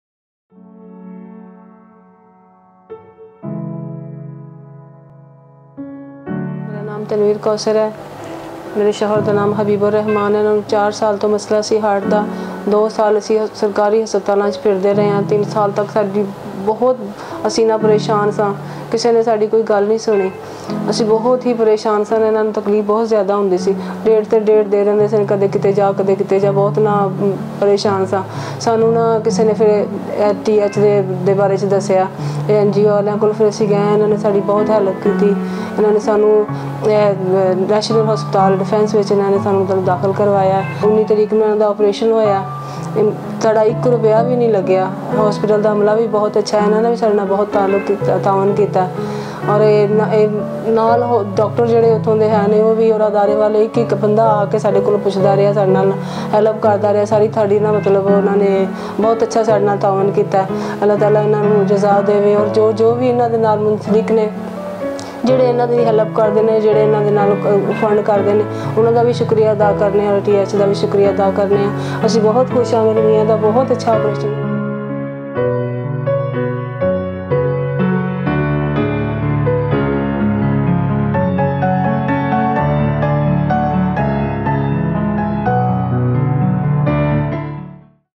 Habib ur Rehman's Successful Open Heart Surgery | Chest Pain | Types of Open Heart Surgery
Habib ur Rehman, 56 years old, experienced repeated attacks of severe chest pain, which were treated by medical management. His angiography was done and he was diagnosed with a case of triple vessel ischemic heart disease. Habib was advised to open-heart surgery - a coronary artery bypass graft (CABG). His surgery was done at National Hospital, on 17-11-19, by Dr. Ahmed Shehzad. The operation was successful and he was discharged in good health.